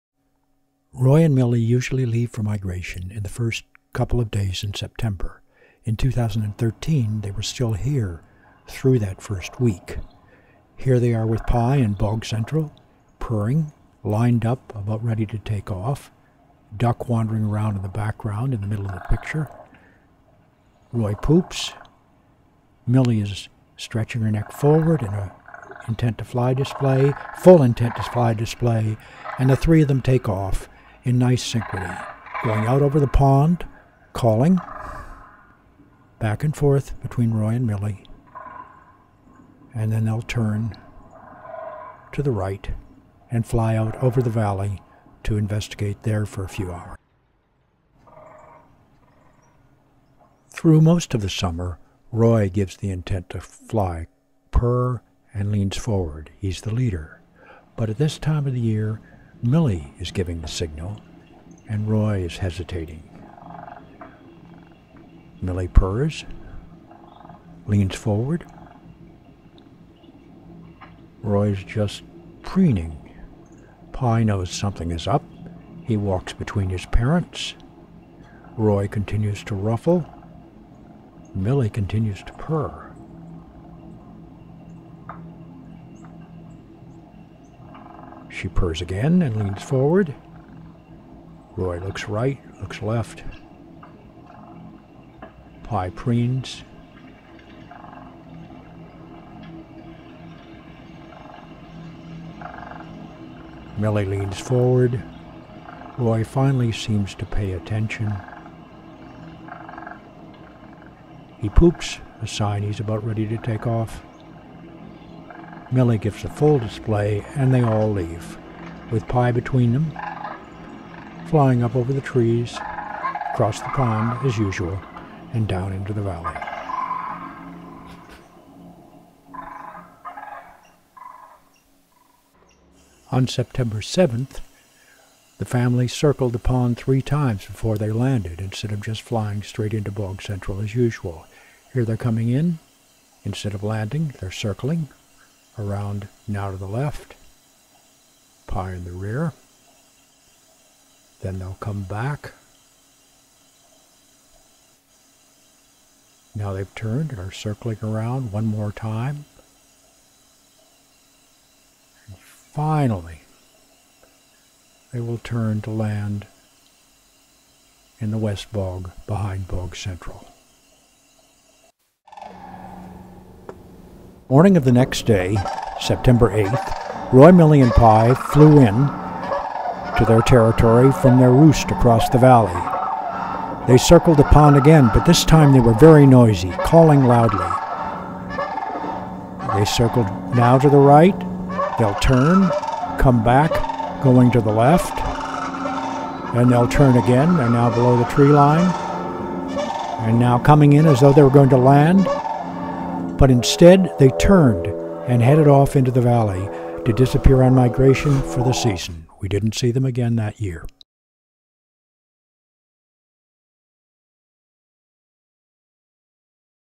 Alaska Sandhill Crane family prepares to migrate
In early September, time was near for departure on the long flight to west Texas. On September 4th, Millie seemed more motivated than Roy to take wing. In the second video below, she purred insistently while awaiting Roy's commitment to fly.

Over the succeeding days as the family returned from excursions, they took to calling energetically and circling the pond 2-3 times before landing.

The four video clips show: 1) a typical August take-off, 2) a take-off on September 5th, when Millie was eager and Roy somewhat reluctant, 3) the family landing on September 7th, and 4) the loud approaching family on September 8th as they circled the pond. But they did not land and instead headed south across the valley to start their migration.